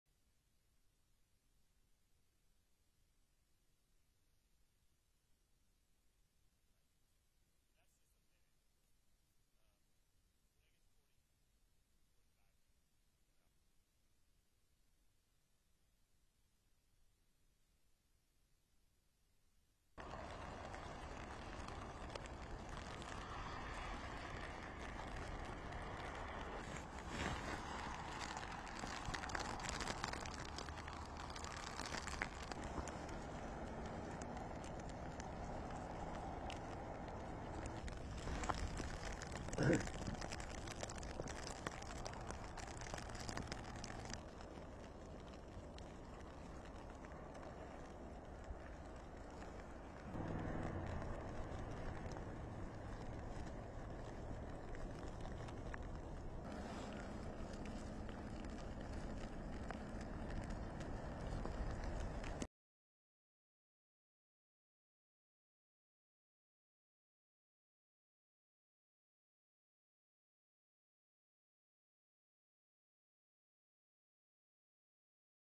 RAW VIDEO: Snow accumulation continues in Lone Tree, CO
Snow falls in Lone Tree, Colorado

Video Credit: The Weather Channel / Date: 03/14/2024